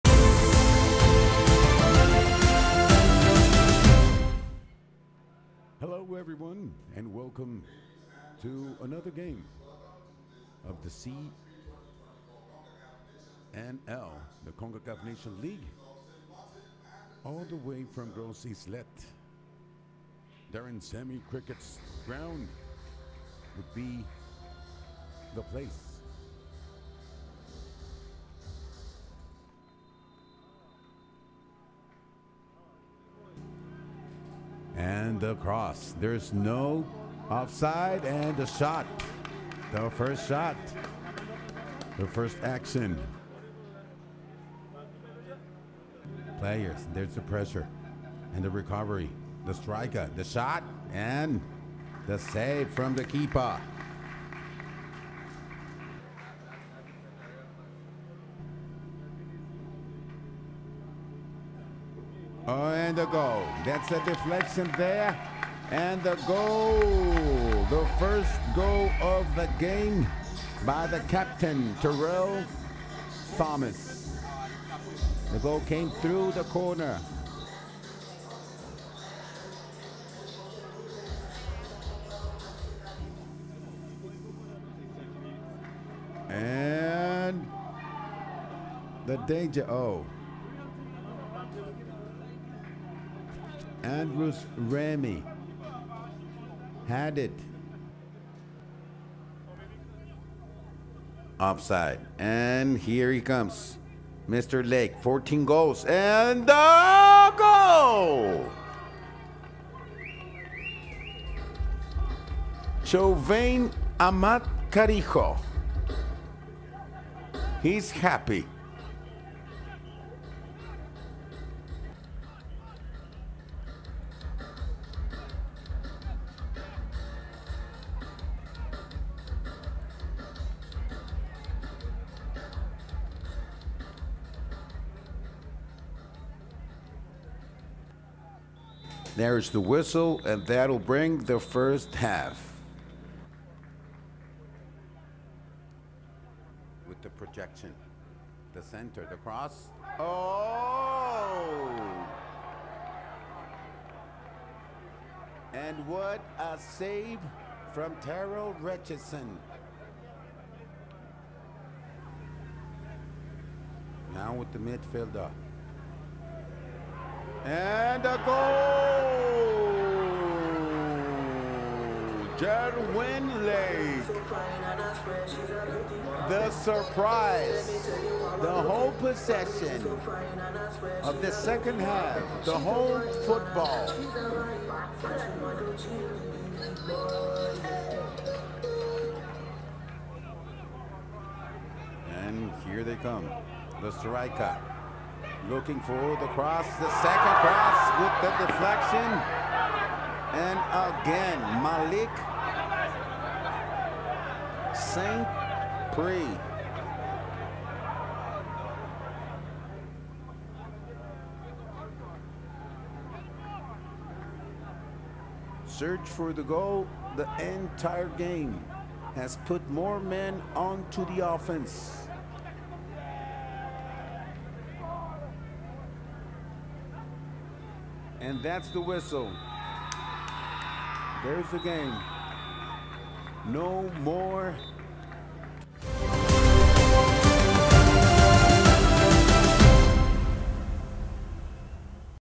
Highlights | Saint Lucia vs Sint Maarten | 2023/24 Concacaf Nations League
Here are the highlights of the St. Lucia vs Sint Maarten thriller 🙌

Sint Maarten took a 2-1 against Saint Lucia at the Daren Sammy Cricket Ground in Gros Islet, Saint Lucia.

The local side was aggressive early on with Caniggia Elva striking in the 3', but Sint Maarten goalkeeper Tyrell Richardson got his hands on it to deflect the shot.

Saint Lucia's Terell Thomas headed in the opening goal in the 17' off a corner kick from Kurt Frederick that took a friendly bounce inside the box.

Sint Maarten striker Gerwin Lake tried the spectacular with an effort from near midfield in the 17' but was denied by the crossbar. Lake had another effort from outside the box in the 23' but was denied by the hands of Vino Barclett.

Sint Maarten finally broke through with the equalizer in the 38', as Chovanie Amatkarijo put the finishing touch on a buildup.

Lake extended the lead for the visitors in the 57' with a dribble into the box and the power hit past Barclett.

Malik St. Prix nearly tied it in the 74' but his attempt in the box went high of the crossbar.

Barclett had a pair of saves to deny T-Shawn Illidge at the 90' and Elmer De Vries in stoppage time.

St. Prix had one last header to equalize, but was denied by Richardson as Sint Maarten secured their stay in League B.